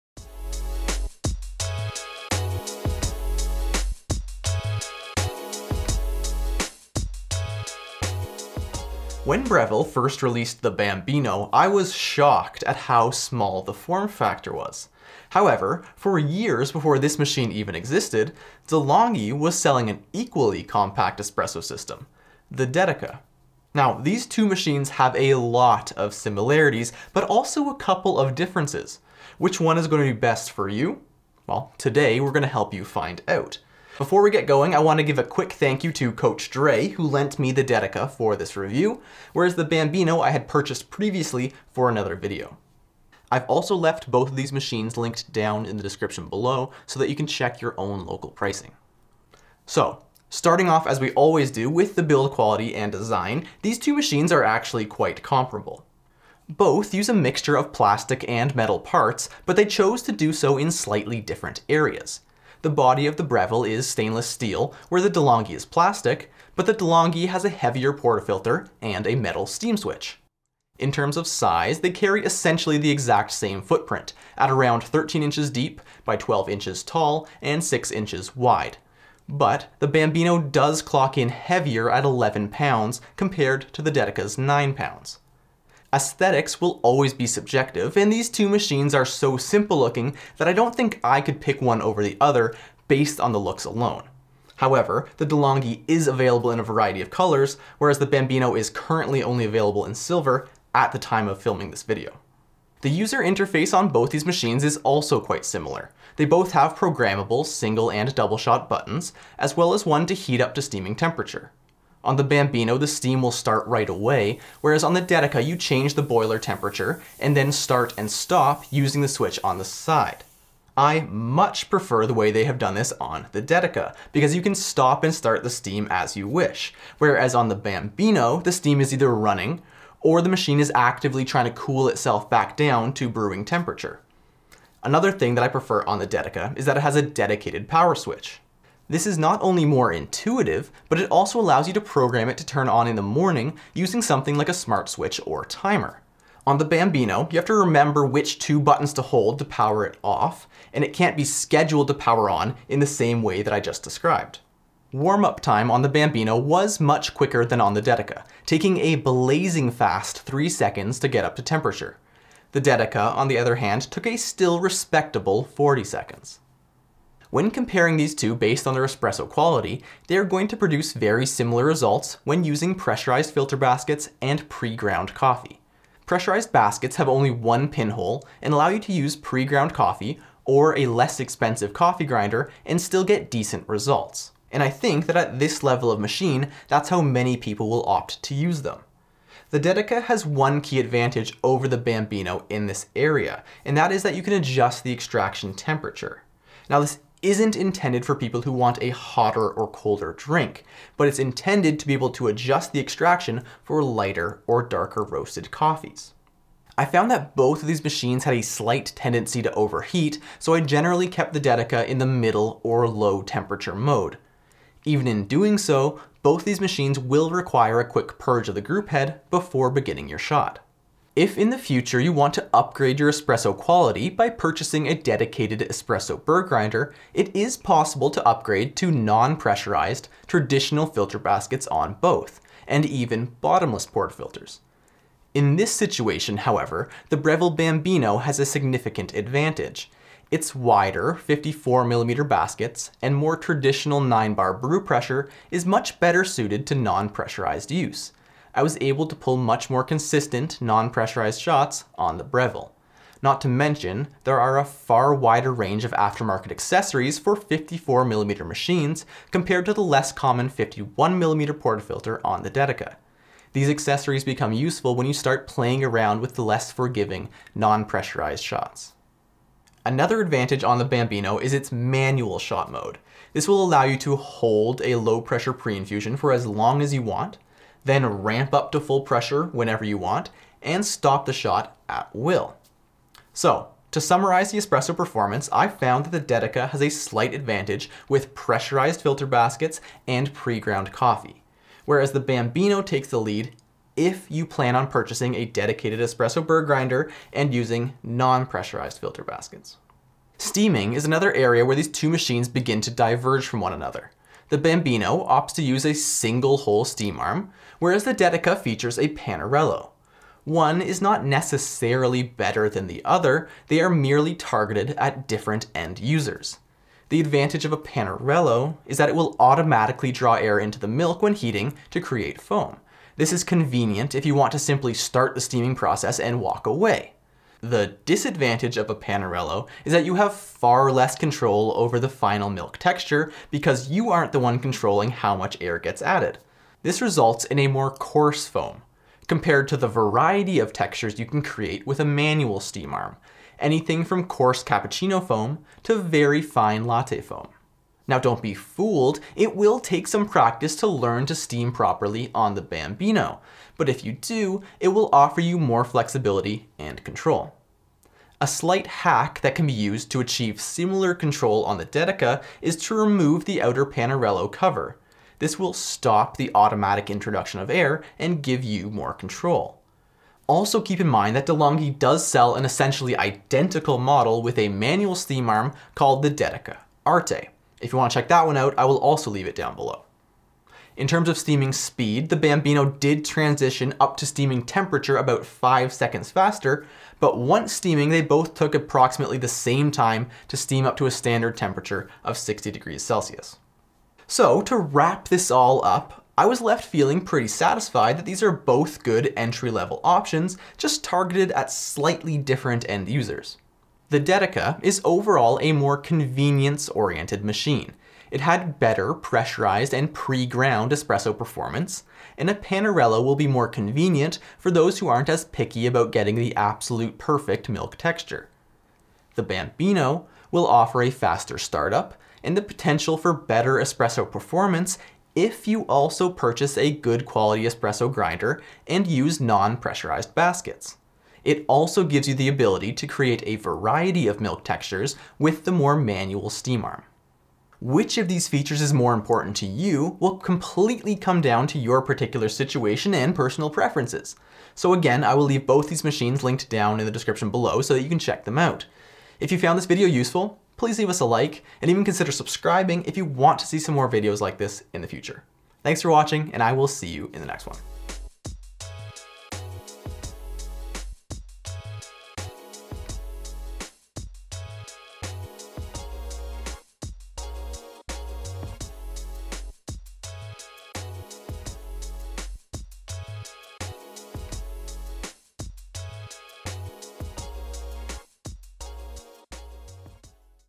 Breville Bambino vs Delonghi Dedica | Compact Kings?
Delonghi Dedica
Breville Bambino
Delonghi Dedica Arte

Breville and Delonghi are often found competing for market share in the home espresso machine segment. And these to compact entry-level machines are no different. The Breville Bambino and Delonghi Dedica are extremely small, but still powerful espresso machines. Which one will be better for you? Today we find out!

0:00 - Intro
0:49 - Build Quality / Design
1:42 - User Experience
2:47 - Espresso Quality
5:00 - Steaming Performance
6:34 - Conclusion